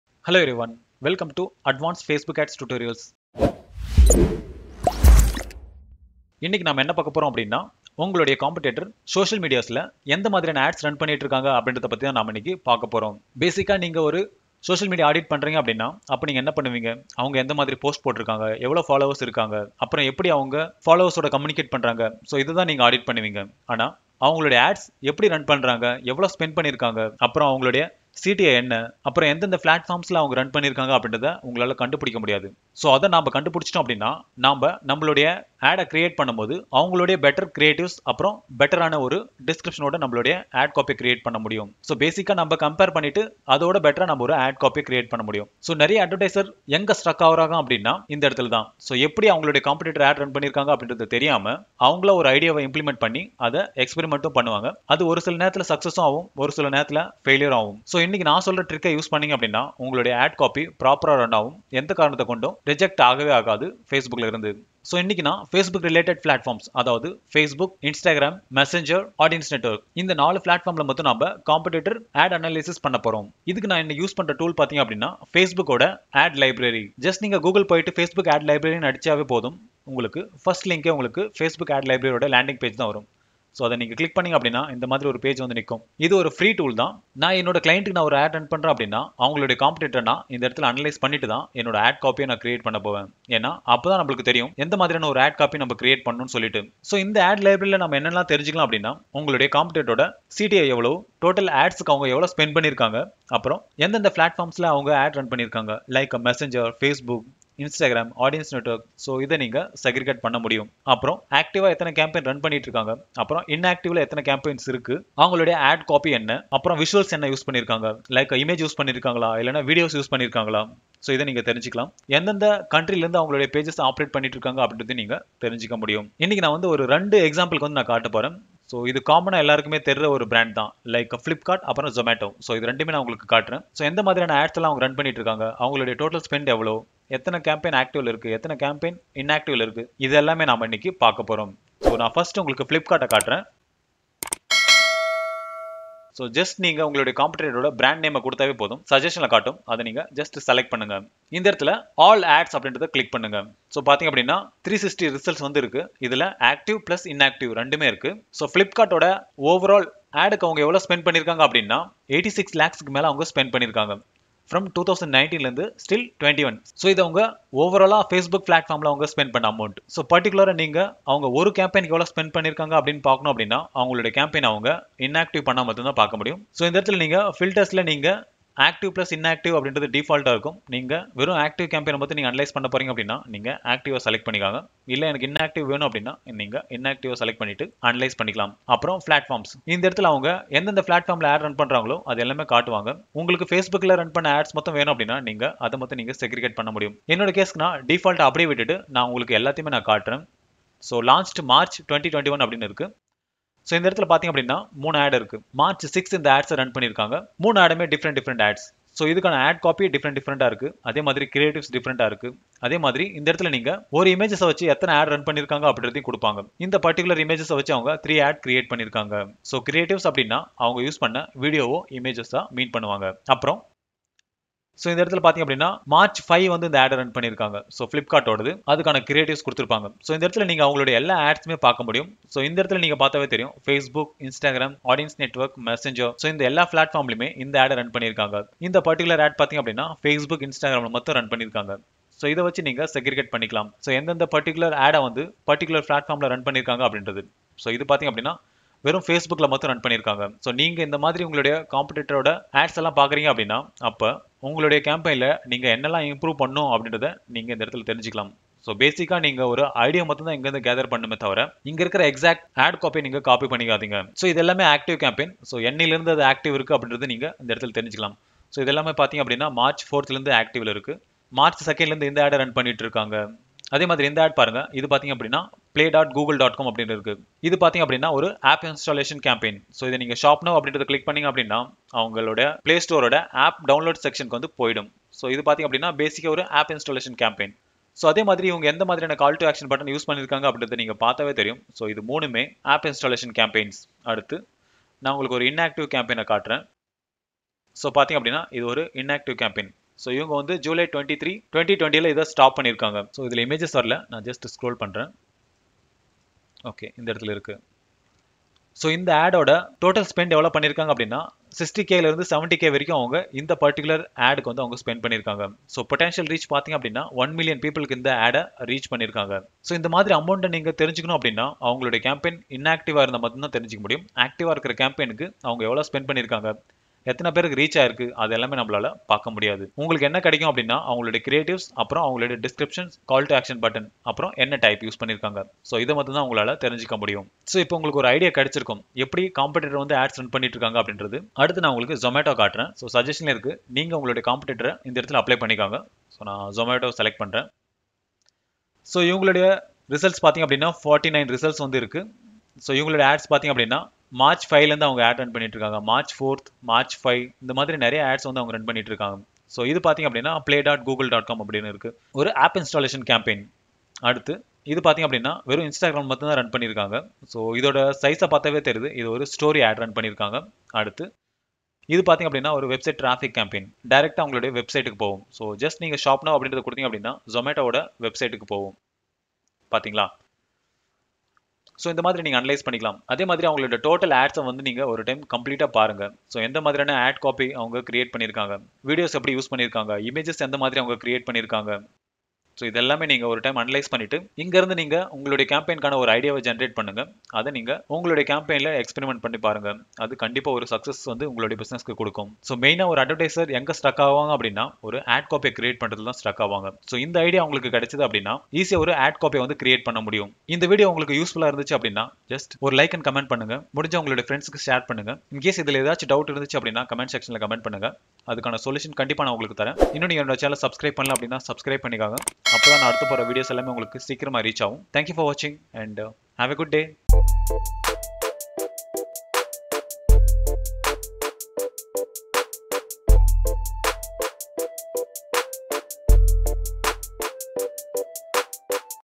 How to Find What Your Competitor is Advertising on Social Media | Social Media Audit in Tamil | #20
In this video, I am going to show you what your competitors are advertising on social media?
The Facebook Ad Library provides advertising transparency by offering a comprehensive, searchable collection of all ads currently running from across Facebook apps and services, including Instagram.